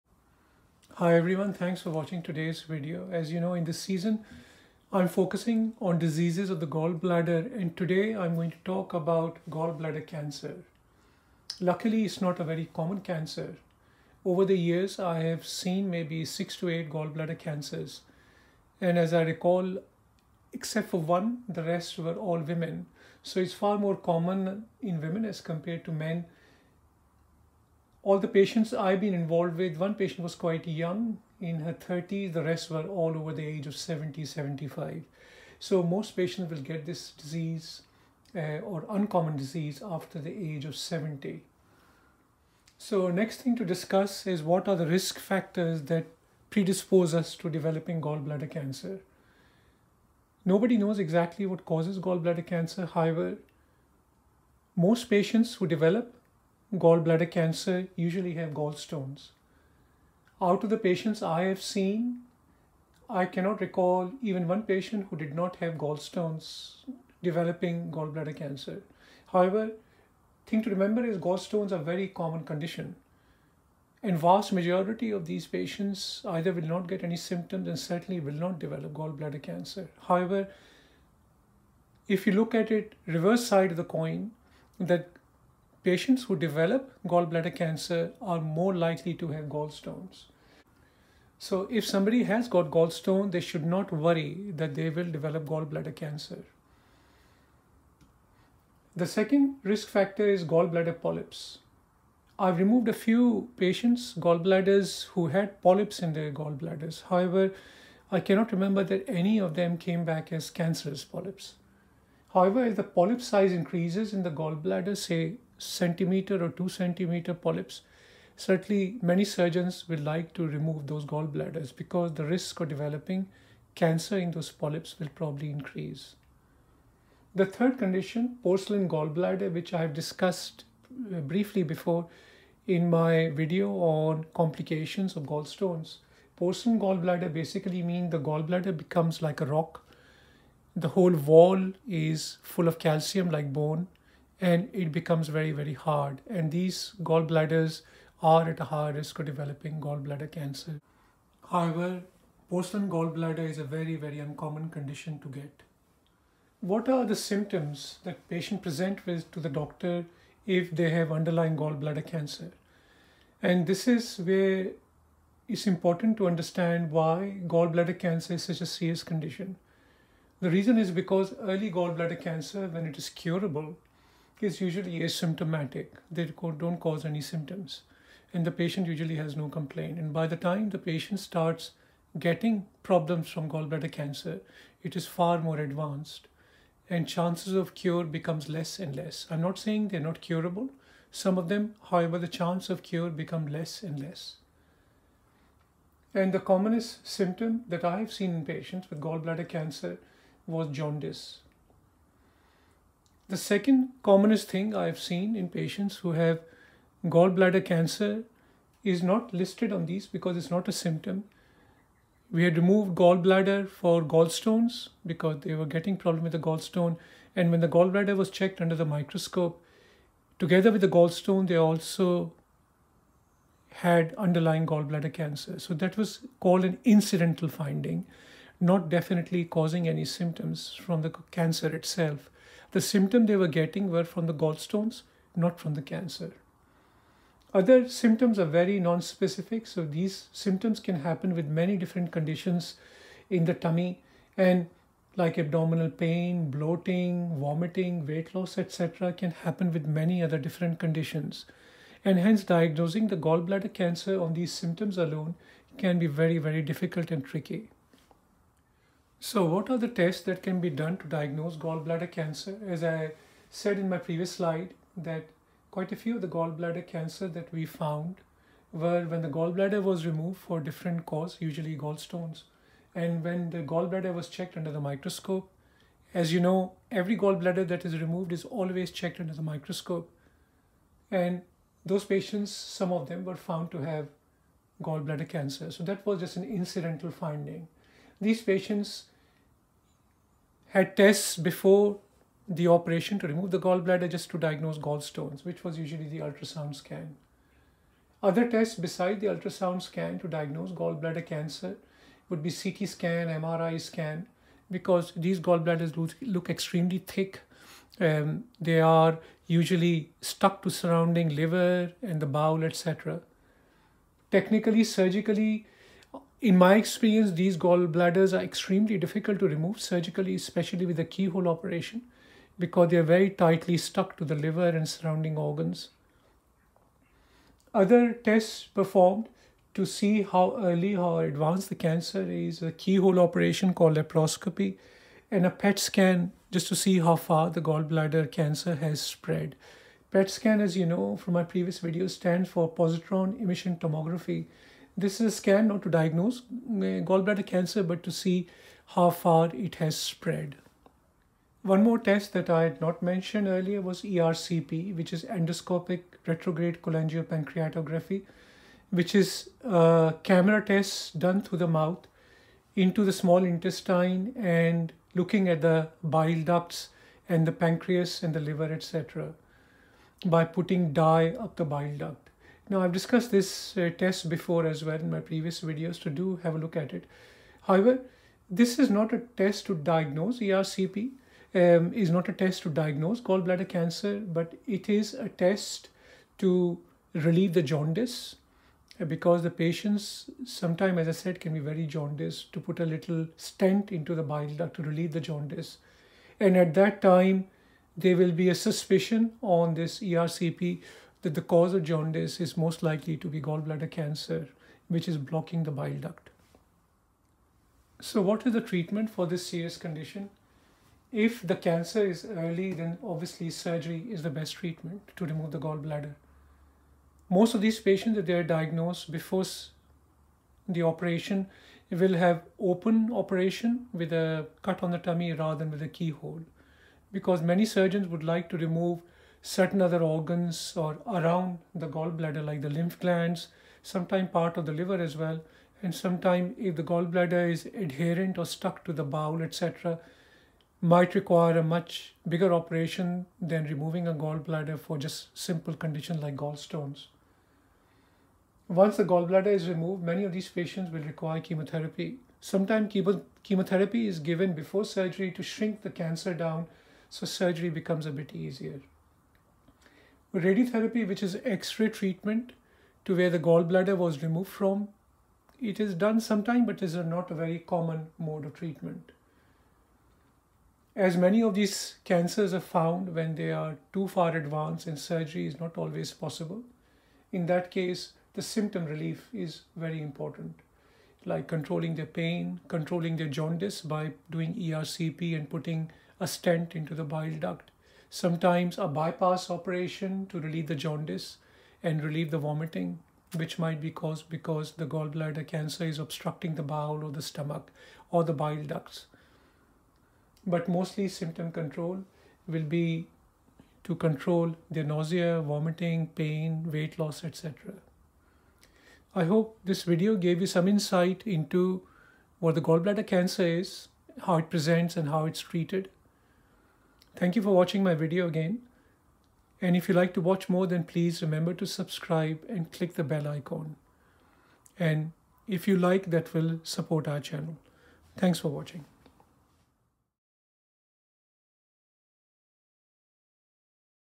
S3E7 Gallbladder Cancer
This video explains about Gall Bladder cancer in simple language